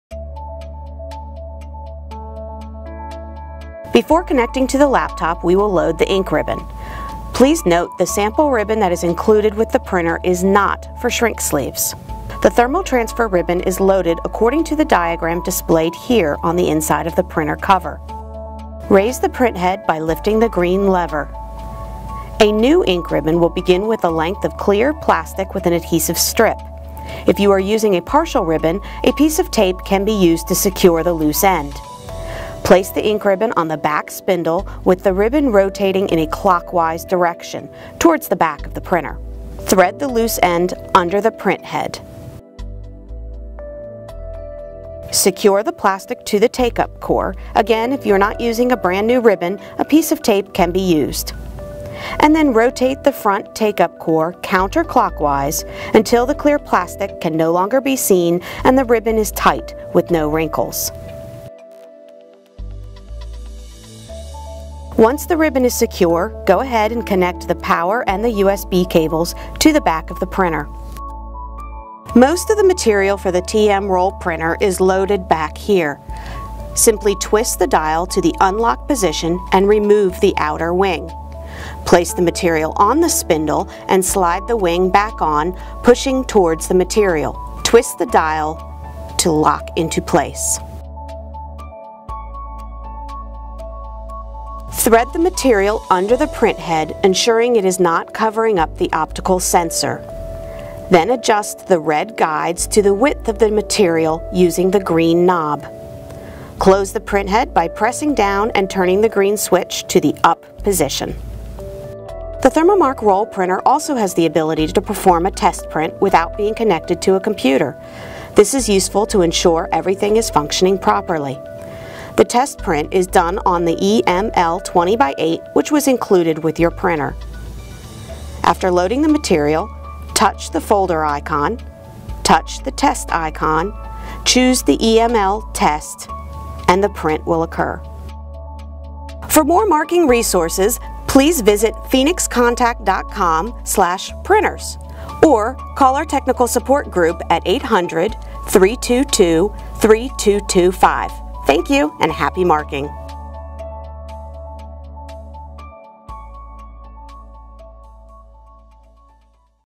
Phoenix Contact's Themomark Roll Printer Setup — Allied Electronics & Automation
The THERMOMARK ROLL 2.0 prints markers in roll format for top-quality terminal, wire, cable, equipment, and plant marking applications. You can easily create high-quality printed labels and markers.

Your advantages
The THERMOMARK ROLL 2.0 prints marking material in roll format. For applications in terminal, wire, cable, equipment, and plant marking.
Printing of punched and continuous adhesive labels, insert labels and shrink sleeves.
Fast, high-quality, and durable printing
Easy and intuitive operation via touch color display
Easy to control with the PROJECT complete software
USB and Ethernet connections